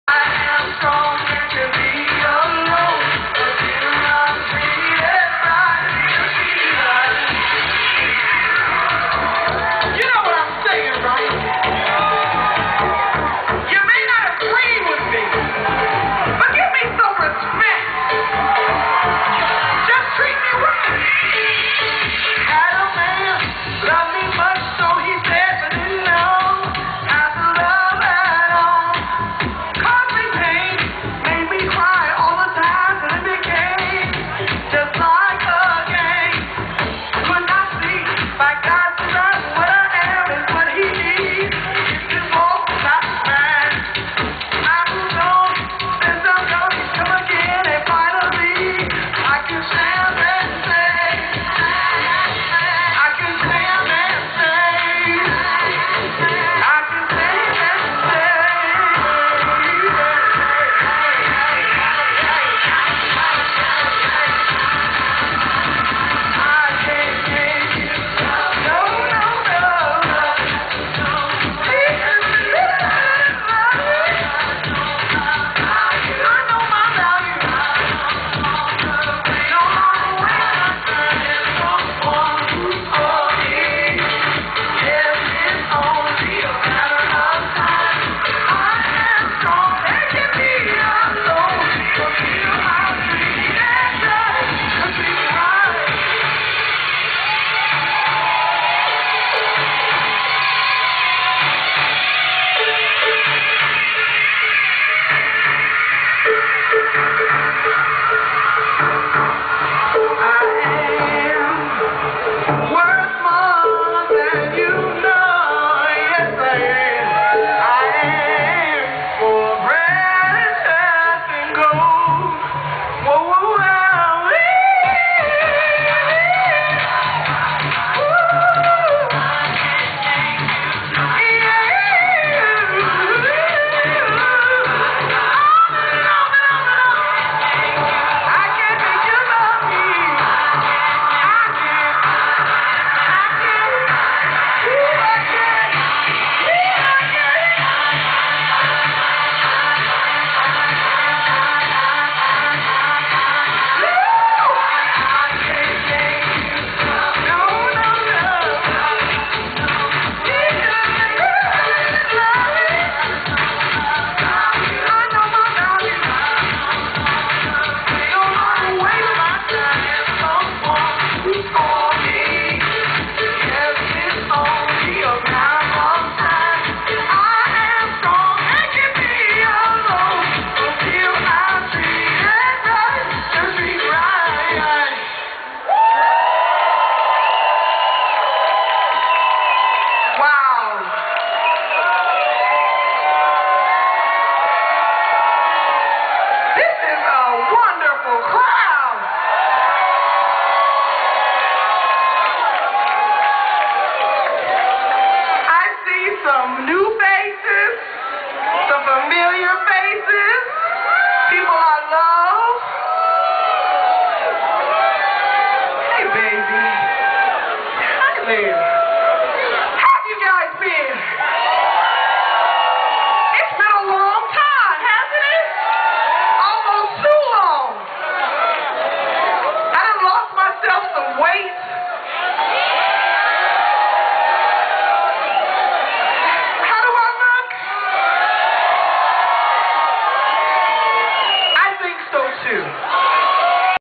Kim English
Kim English...amazing vocalist in Chicago Lakeview fest. voice soaring to the sky!!!